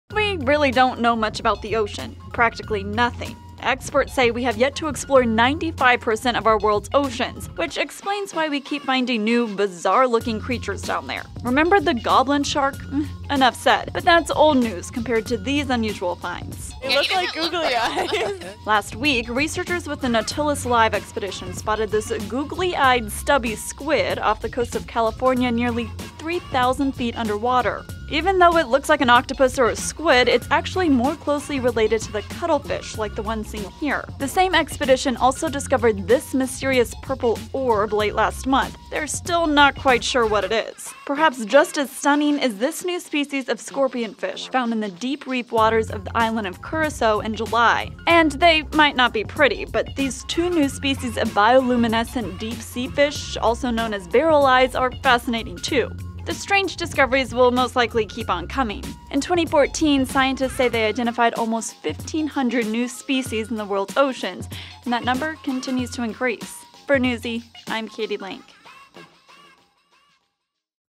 Scientists keep finding odd animals in the ocean
Remember the goblin shark? That's old news compared to these unusual finds.